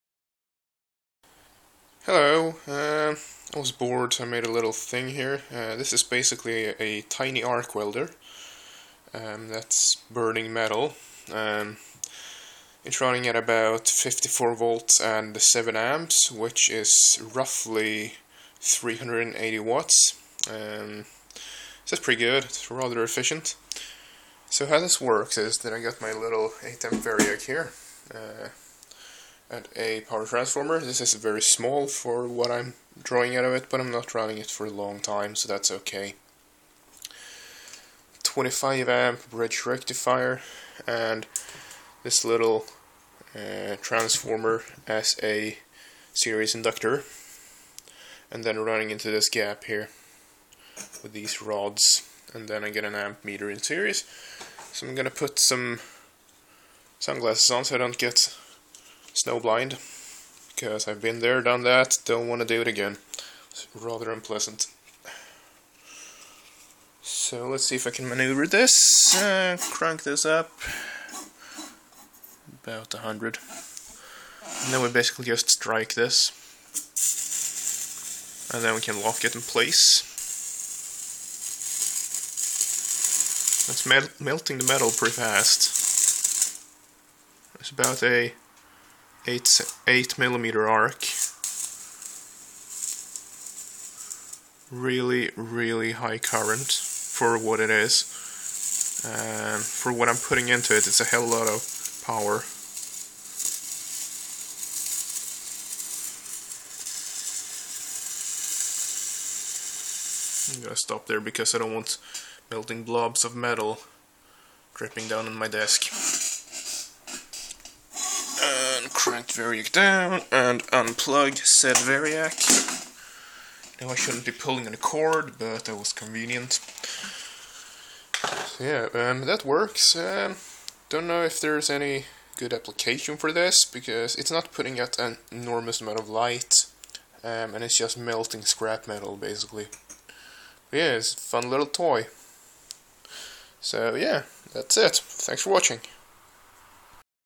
Low voltage high current arc melting metal
A fairly low voltage arc at quite high current melting some metal.